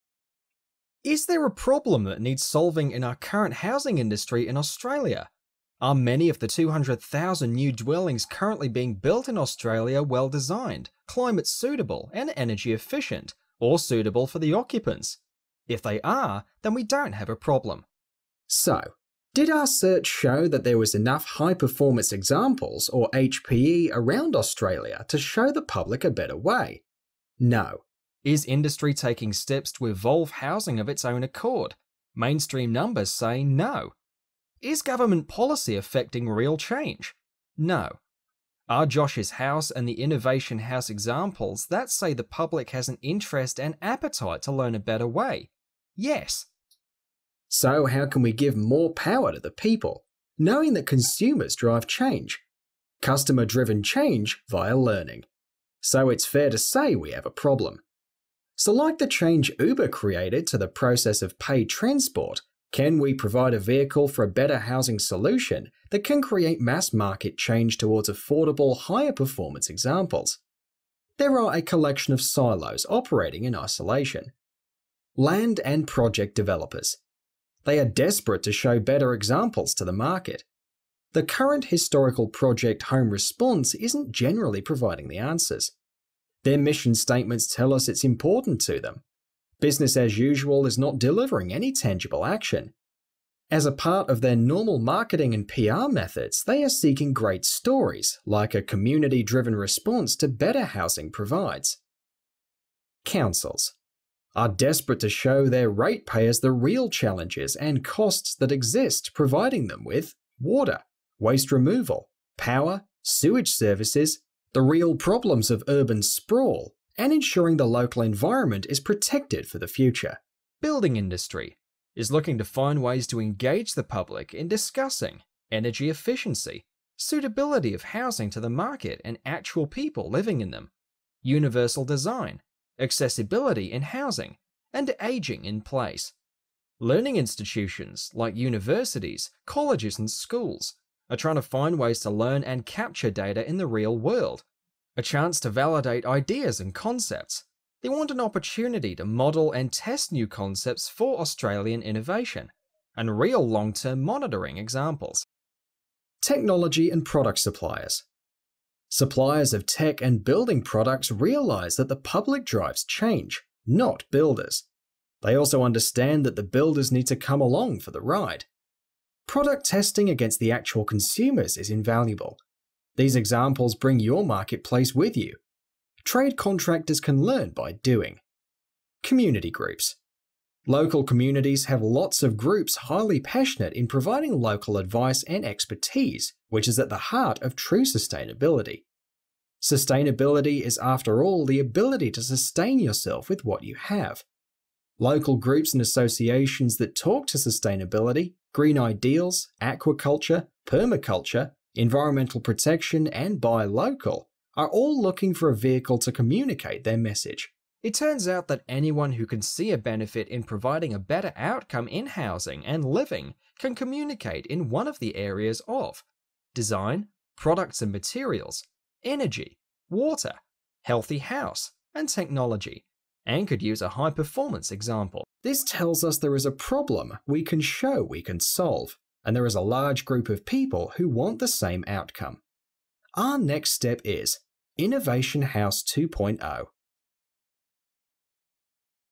High Performance Housing Examples
So what does the future of housing hold.  Is there a better housing solution available and how do we confirm that this is a better option?